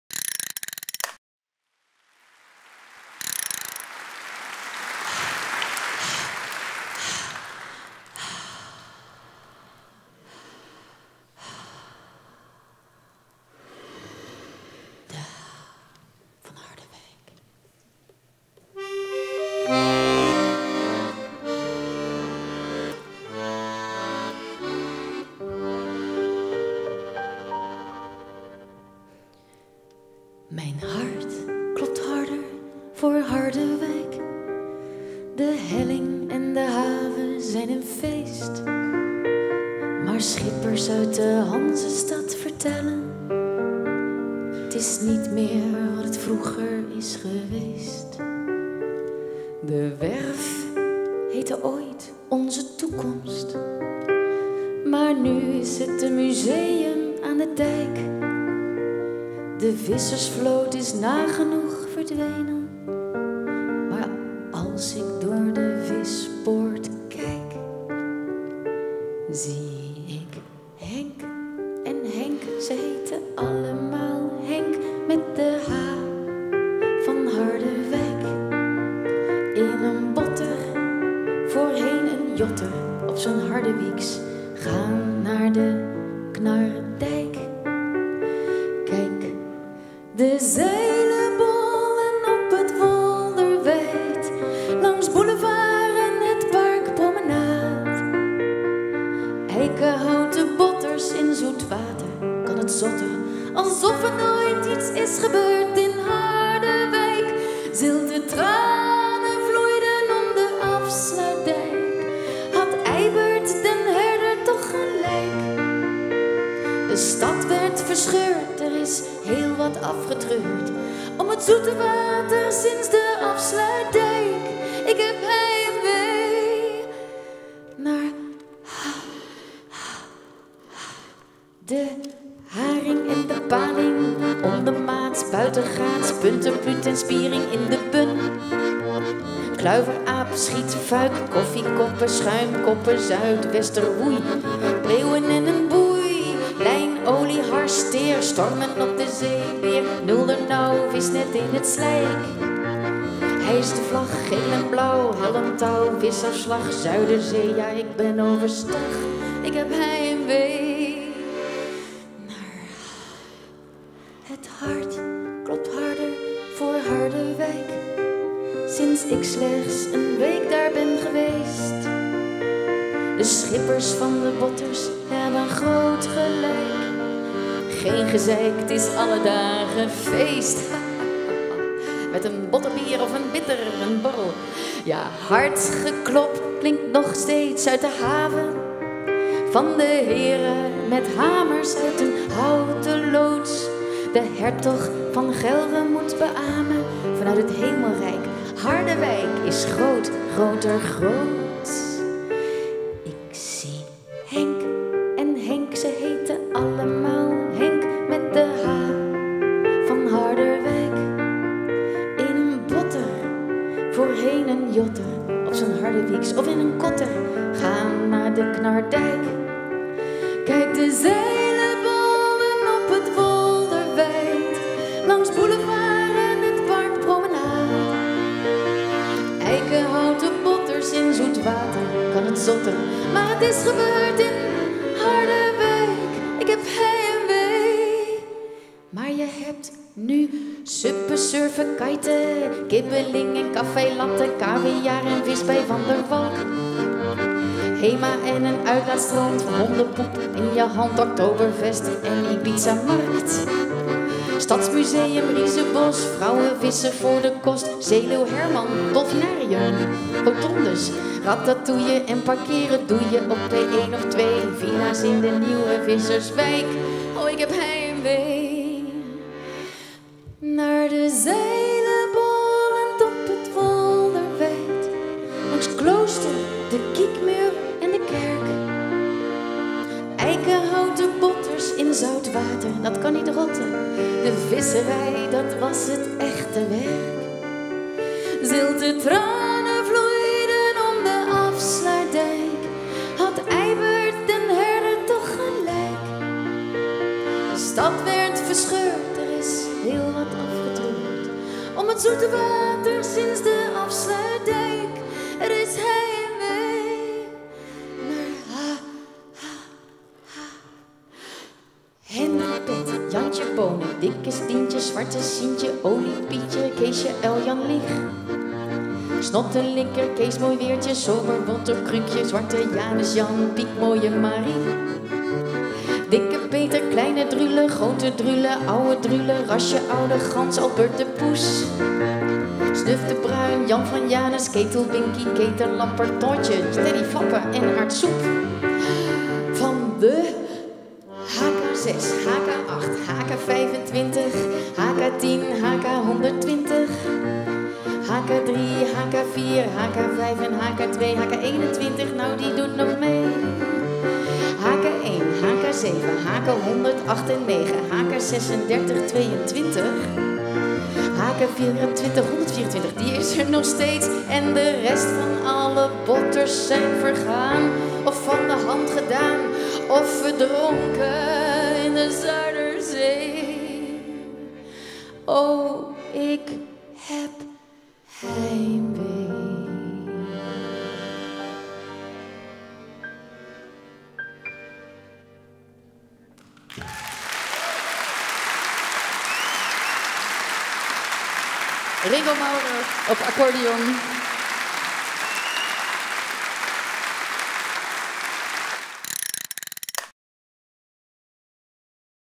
ERFGOEDFESTIVAL 2021 | Ellen ten Damme
Ellen ten Damme, zangeres en muzikant, ging voor het Erfgoedfestival naar het Bottermuseum in Harderwijk. Zij liet zich hier inspireren door het rijke vissersverleden van de stad. Ze ging uit varen op een botter en werd rondgeleid langs de havens. Na afloop van haar residentie schreef Ellen een prachtig lied, waarin ze zingt over haar heimwee naar Harderwijk. Dit nummer liet ze horen tijdens de opening van het Erfgoedfestival.